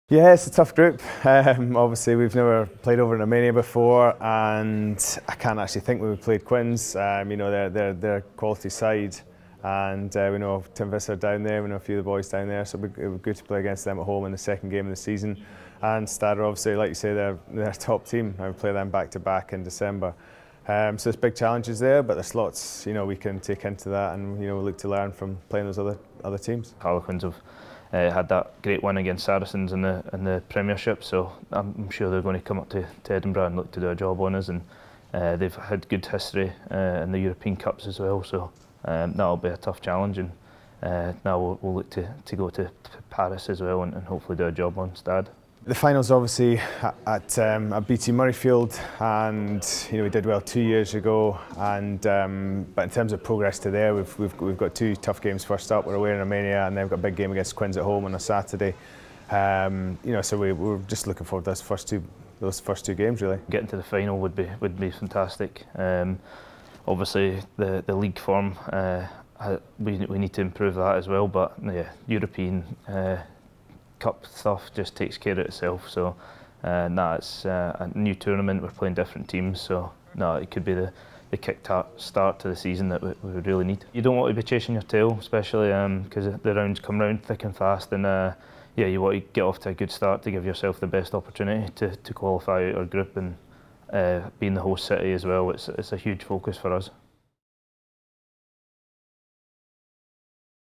EPCR | Home Challenge Cup final a real motivation for Edinburgh
Edinburgh head coach Duncan Hodge and captain Duncan Weir insist the chance to play a Challenge Cup final at their home stadium will be a hugely motivating factor in this year's competition.